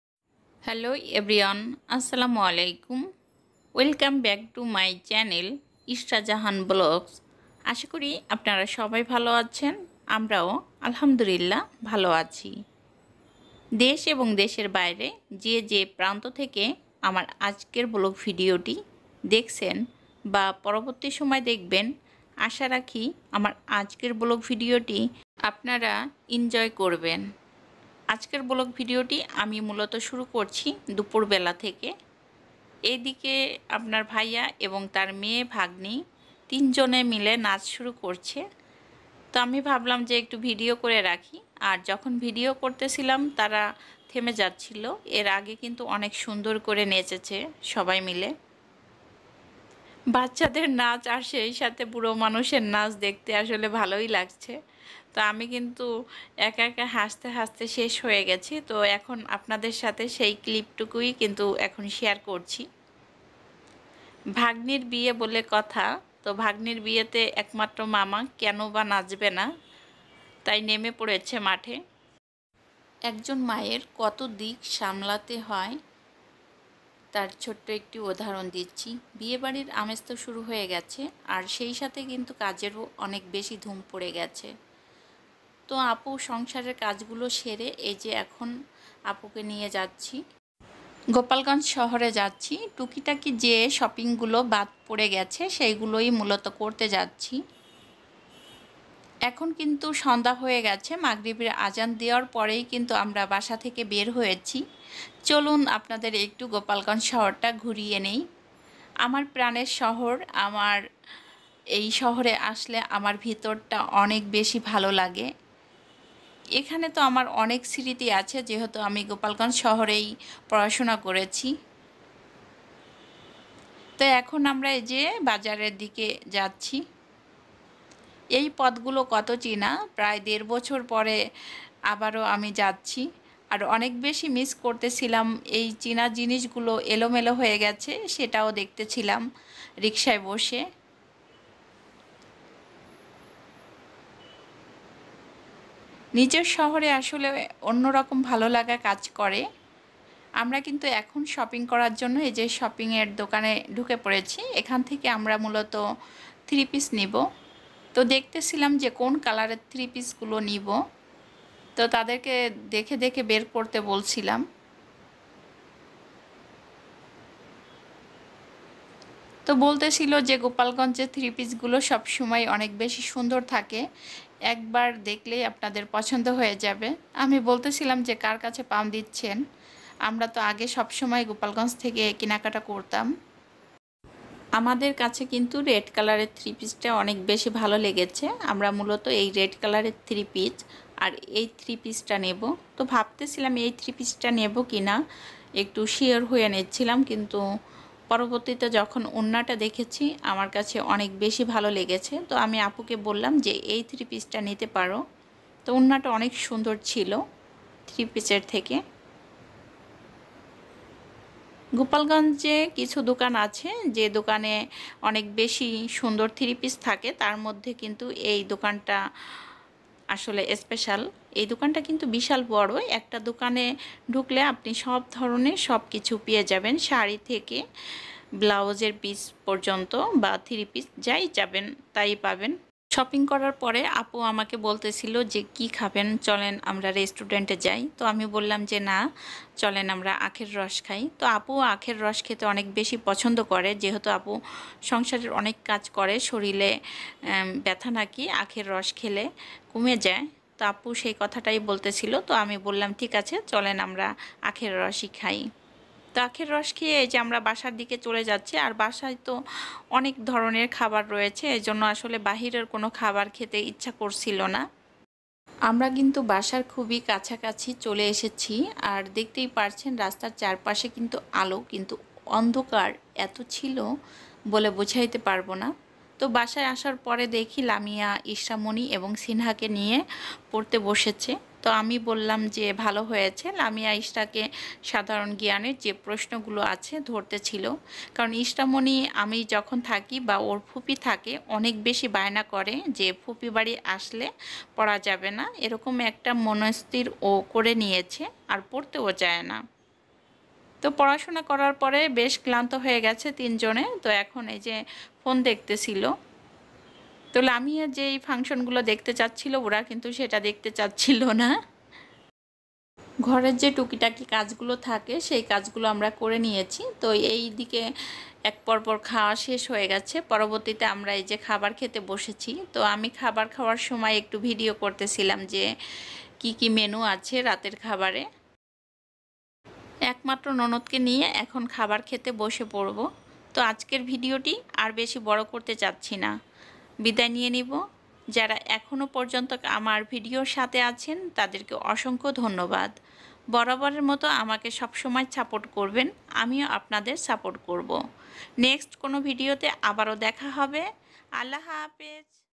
তাজিনের বিয়ের আনন্দ শুরু হয়ে গেছে|| বিয়ের কনের জন্য শপিং করতে বের হলাম
তাজিনের বিয়ের আনন্দ শুরু হয়ে গেছে। বিয়ের কনের জন্য কি শপিং করতে বের হলাম।